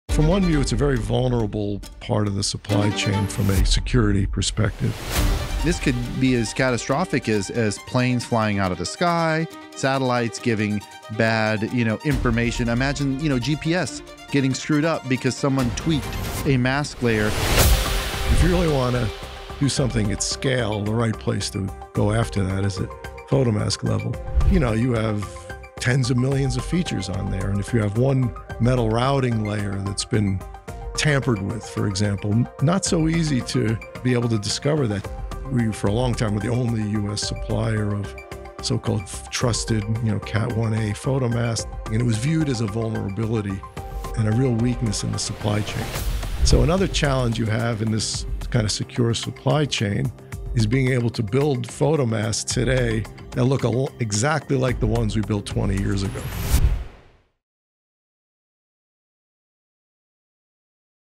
You Could Screw Up Satellites With One Tiny Flaw
🔍 A microscopic defect can bring down satellites, distort GPS, and compromise global security. Discover how an attack at the photomask level could disrupt the entire chip supply chain — and why detecting it is nearly impossible. 🚨